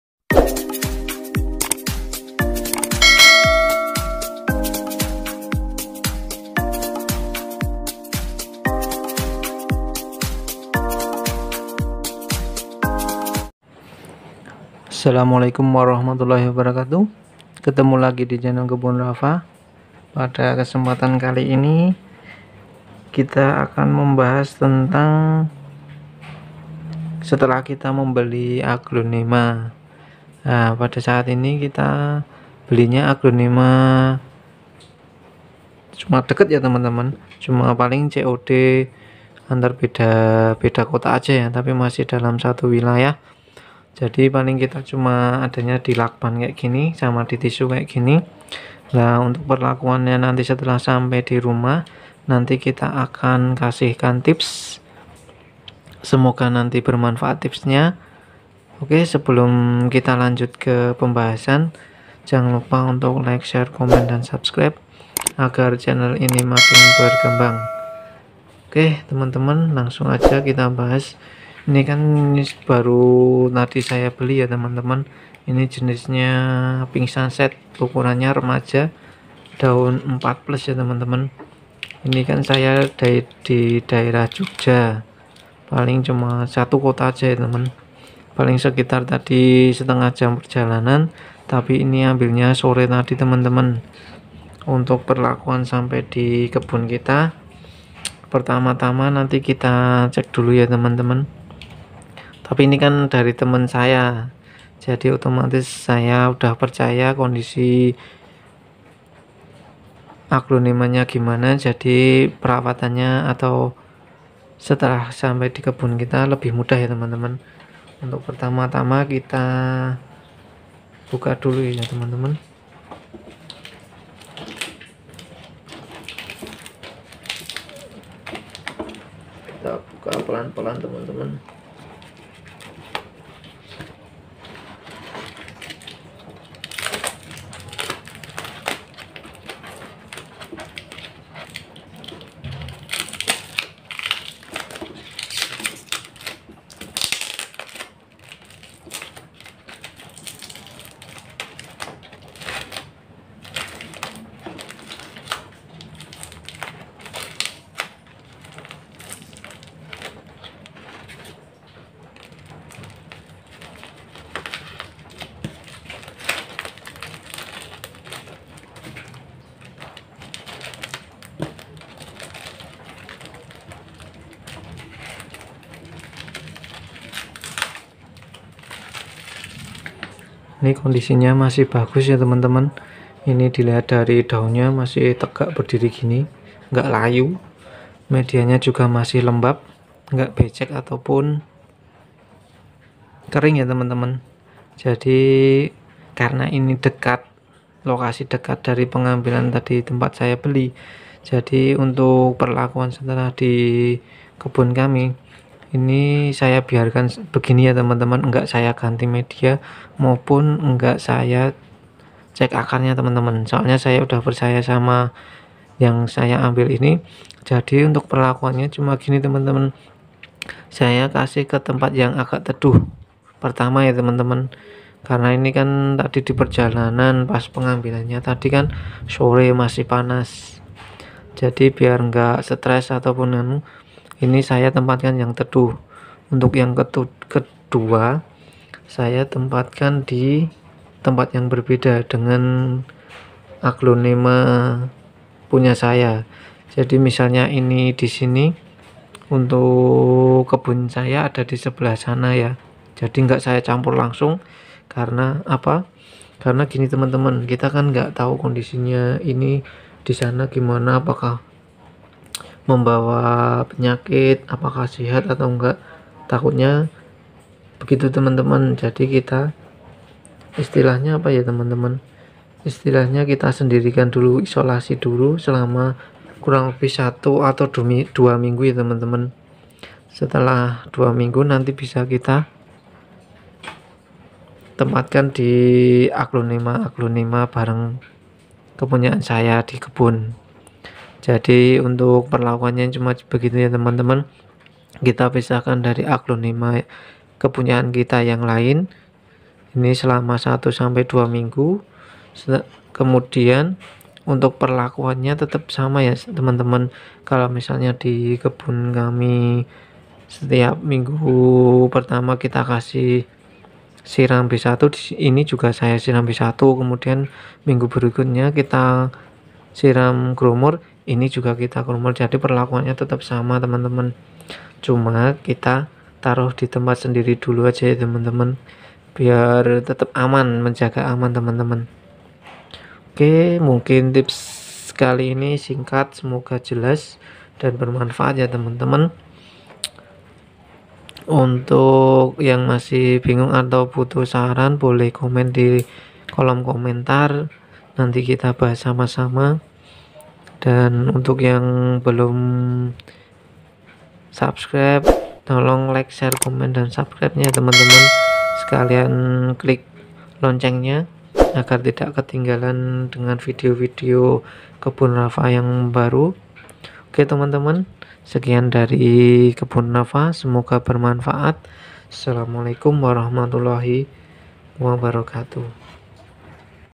Penanganan dan perawatan aglonema baru, beli secara offline
Penanganan maupun perawatan aglonema yang baru kita beli memang perlu banget ya teman-teman, karena pastinya supaya aglonemanya bisa tumbuh subur dan sehat di rumah/kebun kita
Bagaimana penanganannya, simak videonya ya,